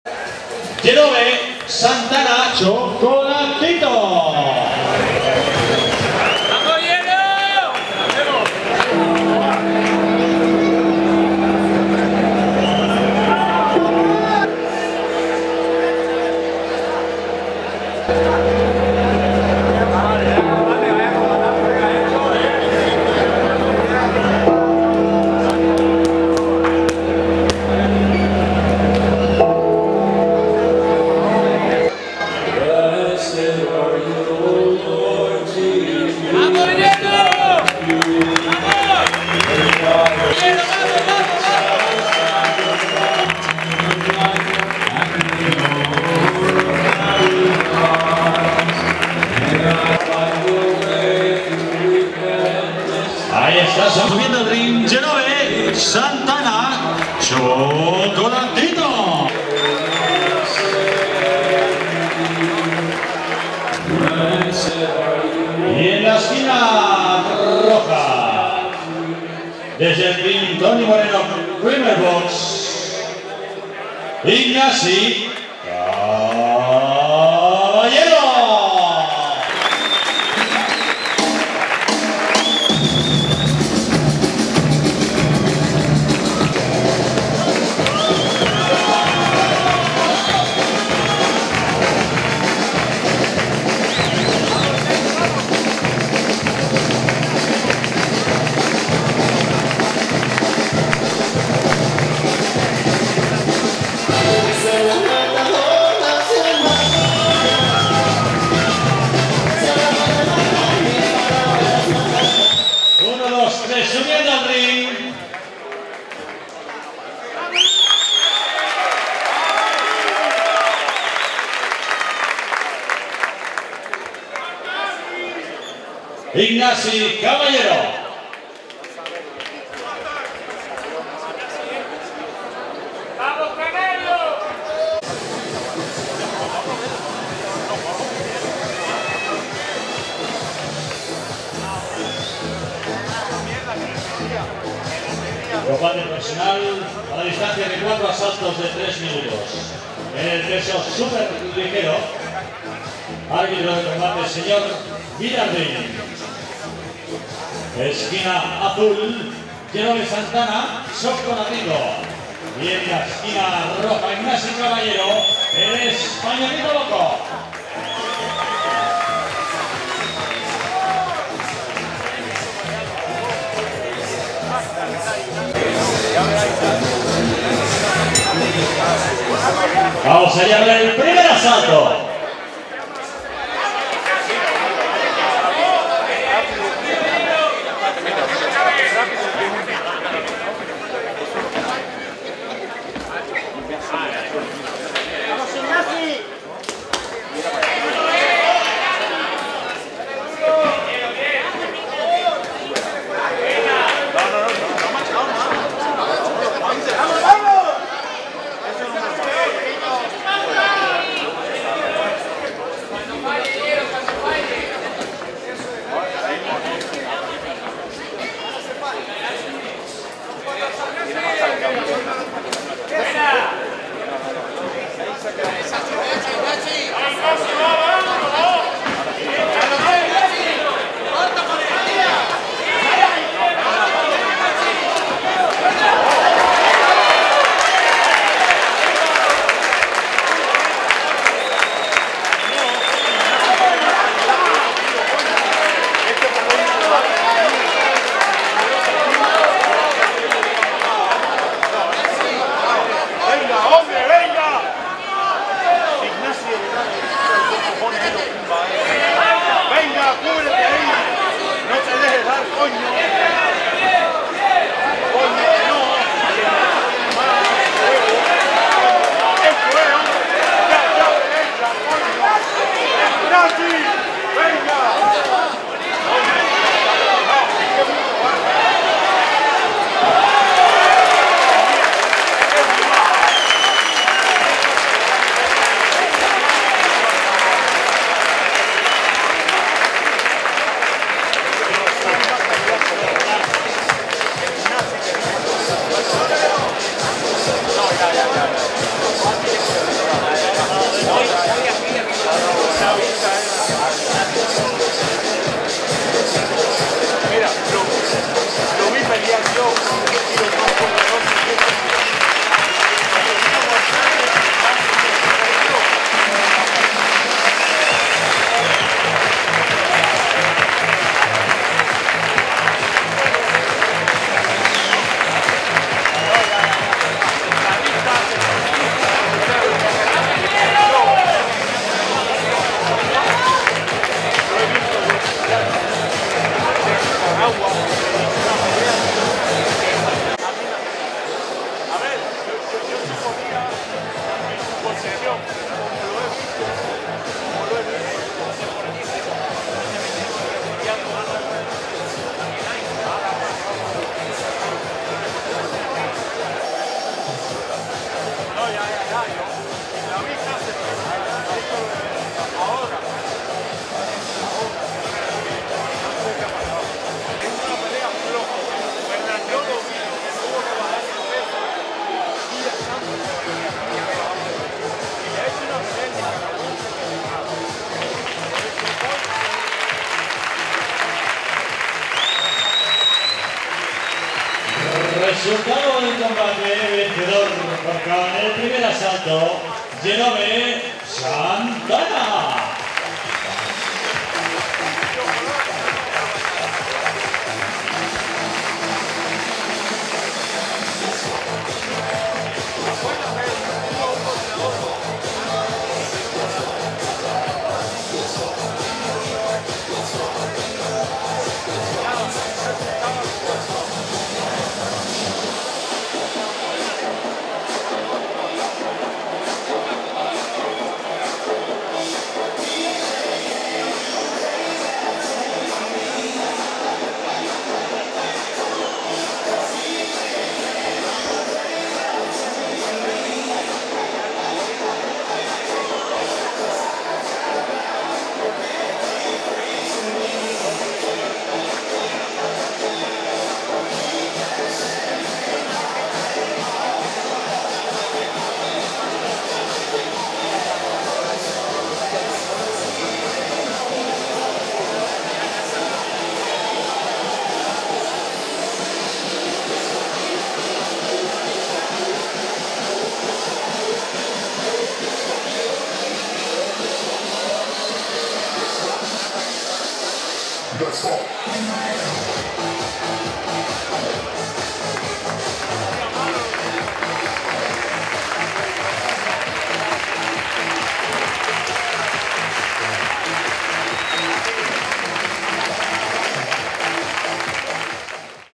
+++Integro +++ Debut Jerobe Chocolatito Santana & Ignasio Caballero. 8 feb 2014, Castelbisbal
El jóven púgil grancanario perteneciente al Team Formento, Jerobe Chocolatito Santana, se desplazó este pasado sábado hasta Barcelona donde debutó como boxeador profesional, de la mano de su entrenador Carlos Formento y de su manager, el conocido promotor Ricardo Sanchez Atocha.

Chocalalito Santana, después de una exitosa carrera amateur con un brillante palmarés, pasó por tanto, con 21 años, al boxeo de pago en la velada organizada Rimer Box y Toni Moreno CBCX en la localidad catalana de Castelbisbal y en la que se enfrentó al experimentado boxeador catalán Ignasi Caballero al que consiguió derrotar dejándolo fuera de combate en el primer asalto.

El catalán empezó el combate imponiendo un fuerte ritmo desde el primer momento con duros ataques al canario de los que supo en todo momento protegerse, comenzando el grancanario a imponer su ritmo y distancia hasta que antes del primer minuto de combate Santana consigue desestabilizar y tumbar al catalán con un duro crochet de izquierda. El catalán pudo recuperarse tras la cuenta realizada por el árbitro, pero ya estaba mermado y antes de finalizar el primer asalto Chocolatito remata la faena con una rápida combinación de golpes que dan nuevamente con el catalán en la lona provocandole esta vez un fuerte KO que obligó a los médicos a subir al ring para reanimar al catalán.

Según su entrenador Carlos Formento "el rival de Jerobe es un peleador experimentado y bastante duro pero nuestro trabajo ya estaba hecho y fuimos a Barcelona a dejar el pabellón de Canarias por todo lo alto. El grado de preparación y concentración de Jerobe era más que excelente y por tanto solo tuvo disfrutar de este momento y demostrar la calidad que tiene encima del ring. Está claro que la carrera de Jerobe como profesional empieza por todo lo alto y estoy convencido que con esta victoria se empieza a escribir la historia de un boxeador del que se hablará mucho y bien dentro de poco". "Chocolatito tiene todas las cualidades necesarias para llegar a ser un gran campeón de las 16 cuerdas", concluyó Formento.